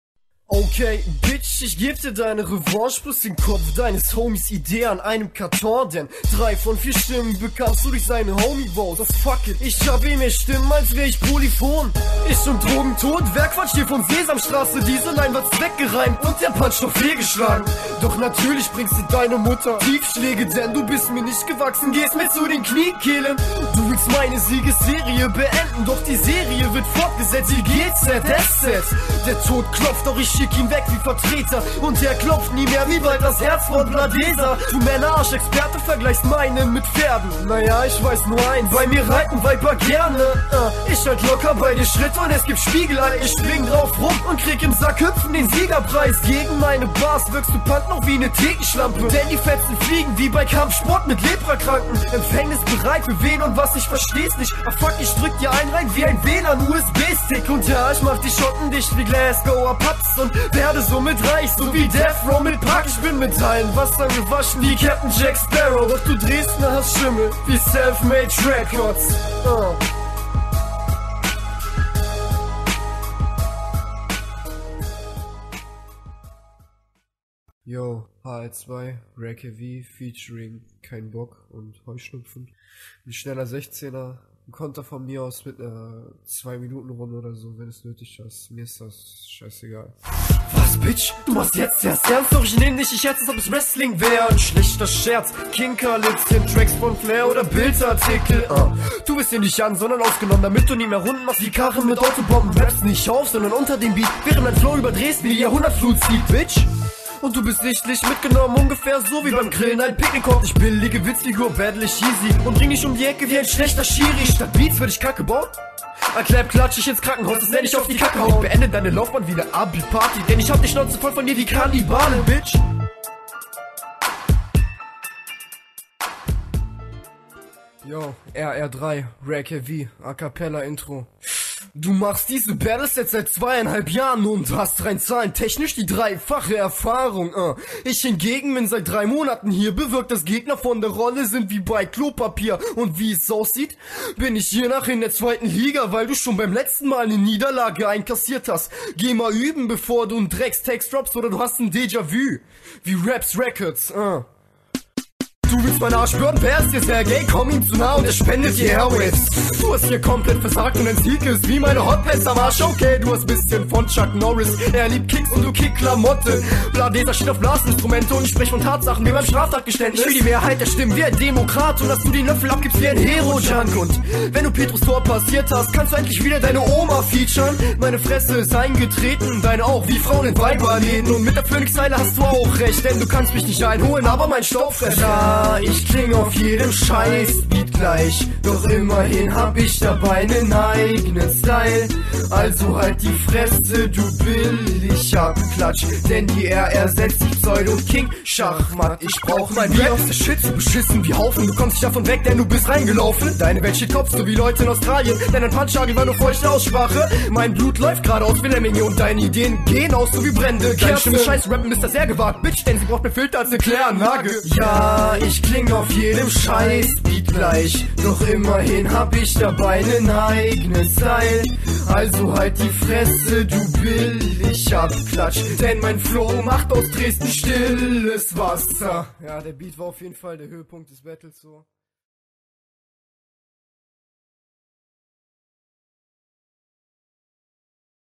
Racaw1 - RBA Battle Nr.6 - VS BlaDesa (zweites Battle/Revanche)
06.2007

Racawis sechstes Battle und das zweite gegen den Forderer BlaDesa.

win - 15:9